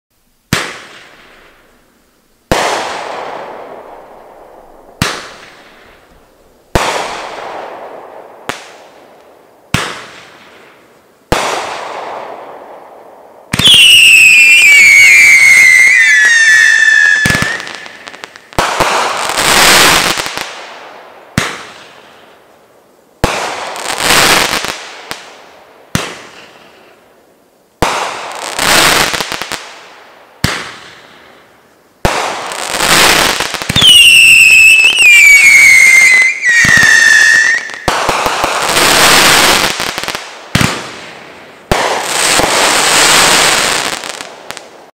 Great Grizzly Fireworks - Alpha 500G
Alpha by Great Grizzly - lasts over 30 seconds and has a great variety of effects. This 25 shot cake features red & blue peonies to white strobe with chrysanthemum crackling, two volleys of exciting 3 stage whistling tails, golden peonies with crackling, and an excellent 3 shot finale of golden crackling palms.